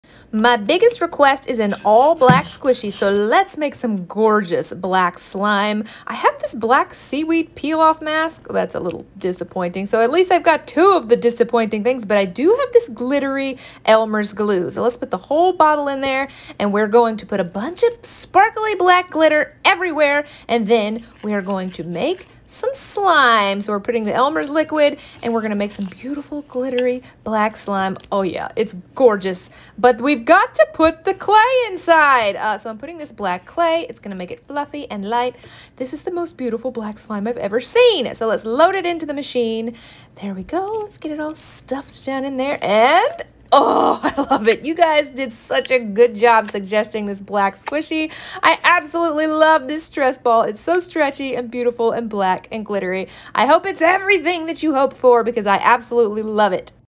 Beautiful All Black Squishy Stress Ball!
One of my biggest requests for a homemade stress ball is all black. So let’s make a gorgeous black squishy in the squishy maker!